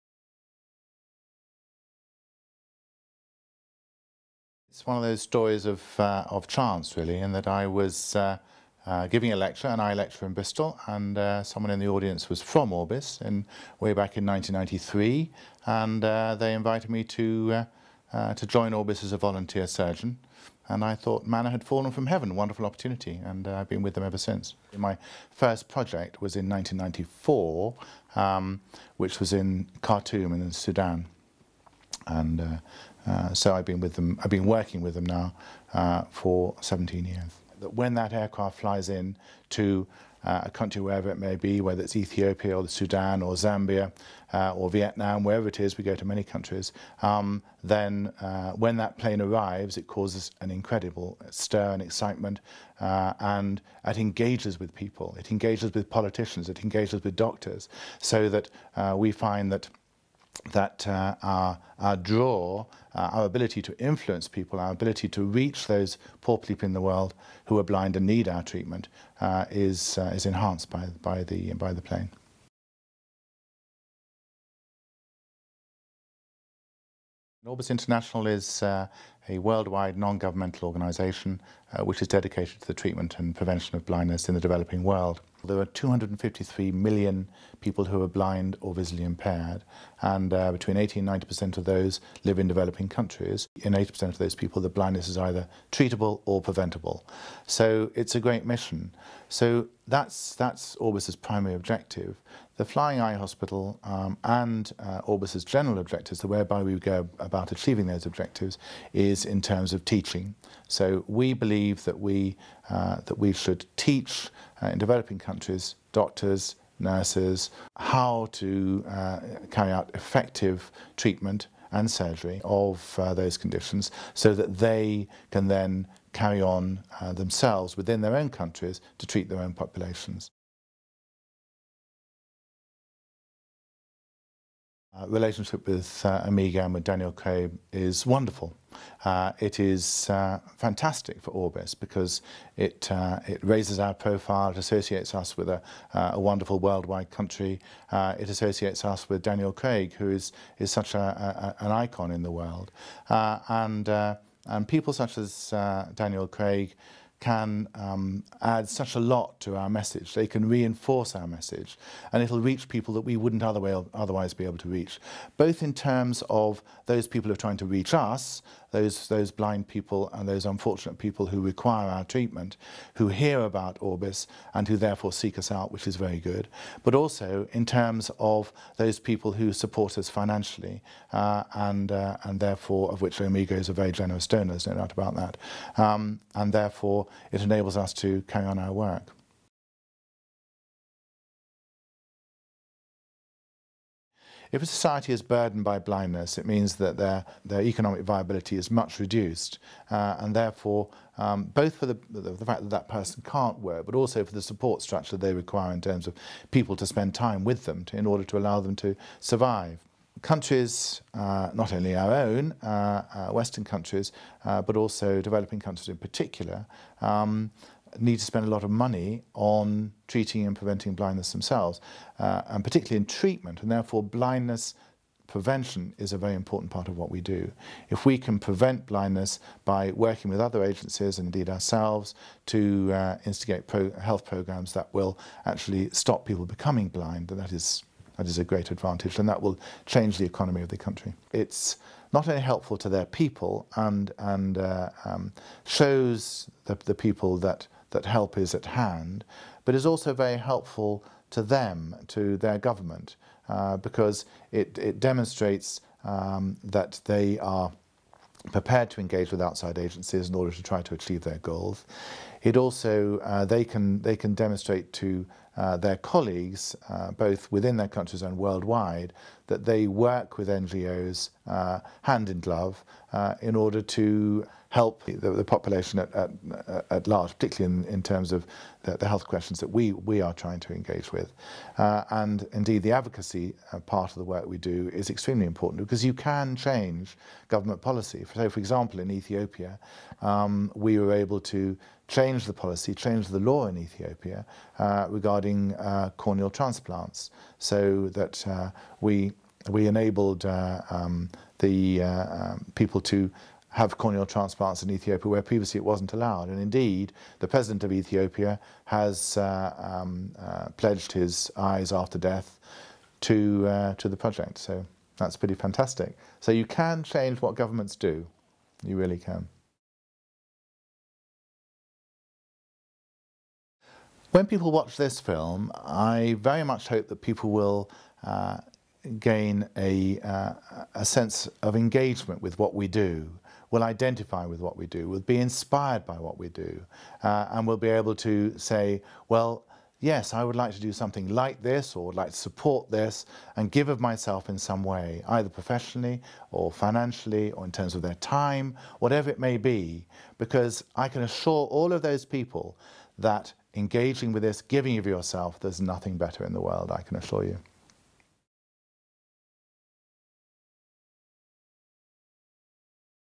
Dr. Robert Walters Talks About ORBIS International | OMEGA
Dr. Robert Walters, Chairman of ORBIS International, describes how he first became involved with the non-profit organisation as a volunteer surgeon and stresses the importance of having OMEGA as a partner. Since its foundation in 1982, ORBIS has implemented eye care programmes to provide training, tools and technology for local partners in 88 countries. The ORBIS Flying Eye Hospital, supported by OMEGA, helps far-flung communities address the tragedy of avoidable blindness. Working together, ORBIS and OMEGA have been able to provide medical care for more than 12 million people and train more than a quarter of a million eye care professionals.